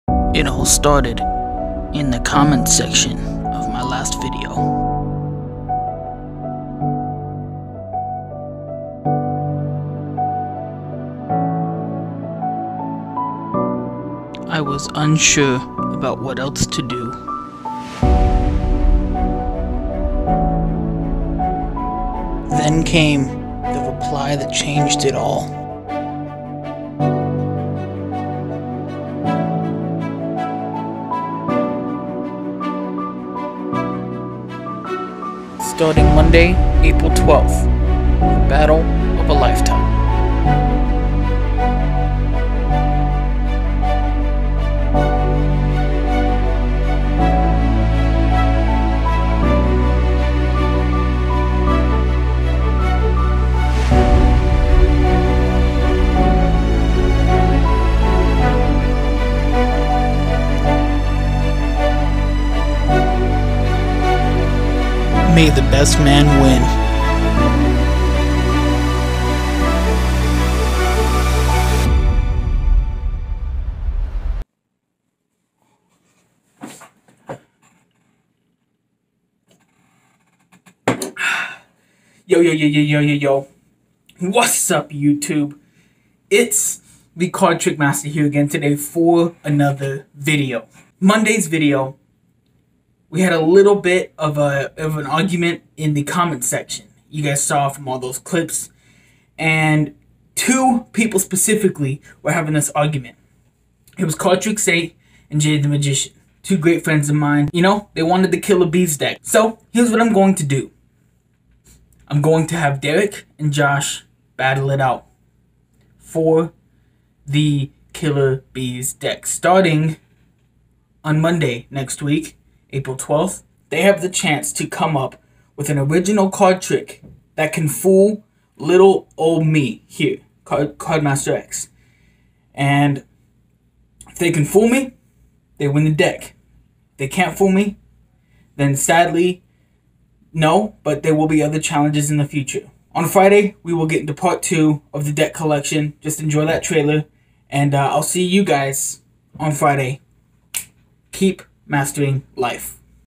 Battle for the Bees [Official Trailer] + Explanation
Enjoy the OFFICIAL TRAILER for CardMasterX' new challenge 'Battle of the Bees.' Two magicians are going to battle it out with card tricks to determine the winner of the coveted Killer Bees deck created and produced by Ellusionist...may the best magician win...